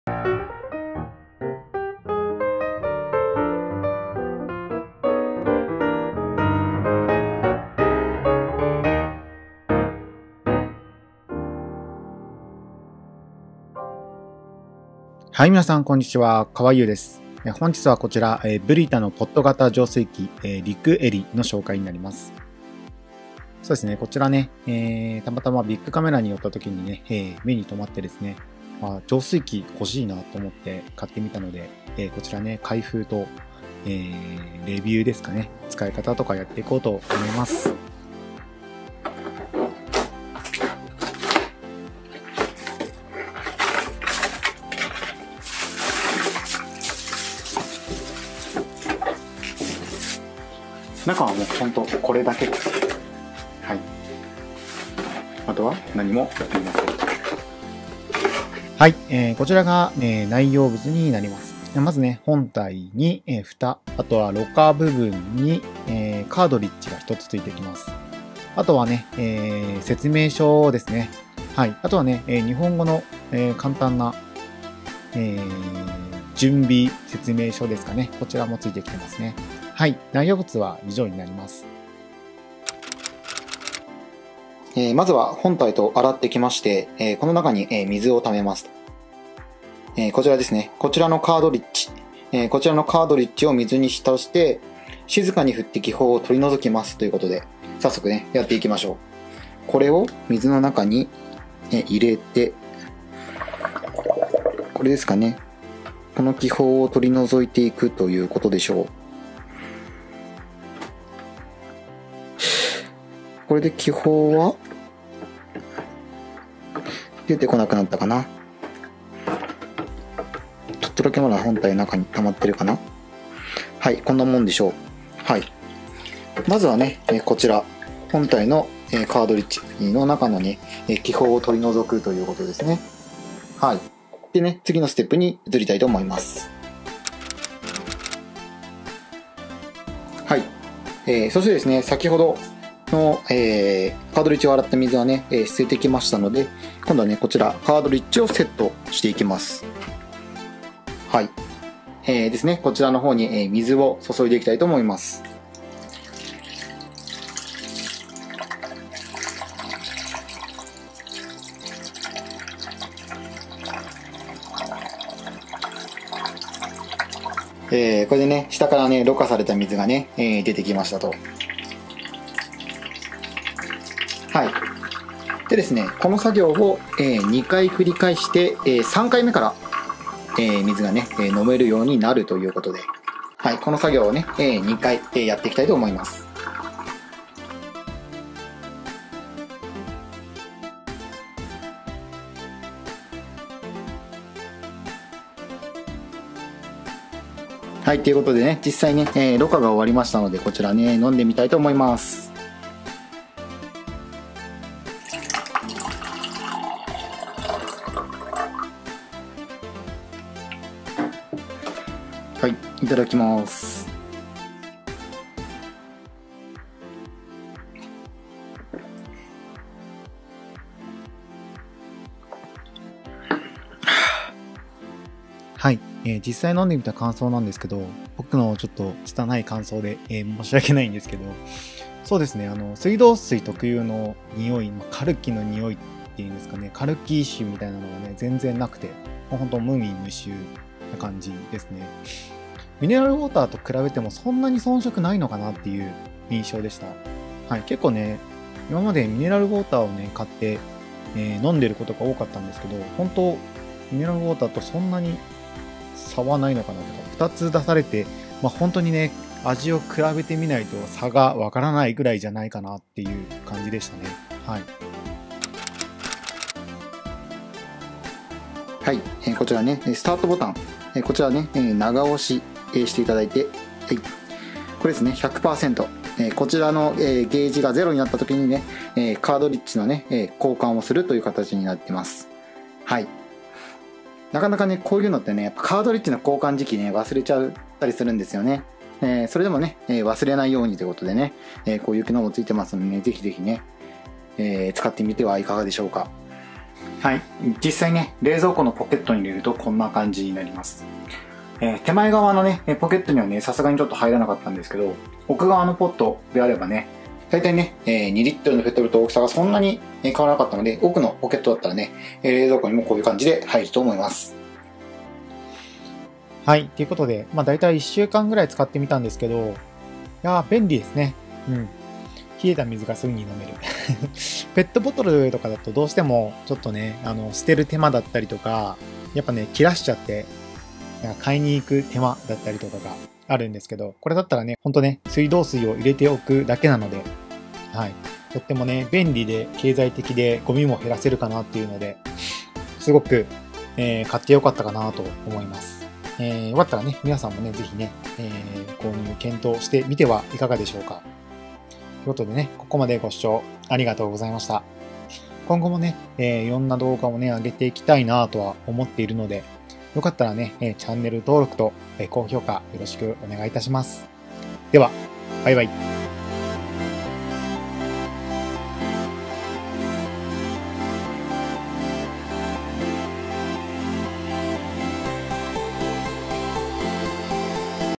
【BRITA】おすすめ！ブリタ ポット型浄水器 リクエリ 美味しい水のある生活をしよう！
□BRITAリクエリ

こんにちは、kawa-yuです。
今回は浄水ポットの紹介です。ミネラルウォーターを買いに行く手間やペットボトルが溜まっていく生活から抜け出したいと思って購入しました。

※製品リンクは、Amazonアソシエイトのリンクを使用しています。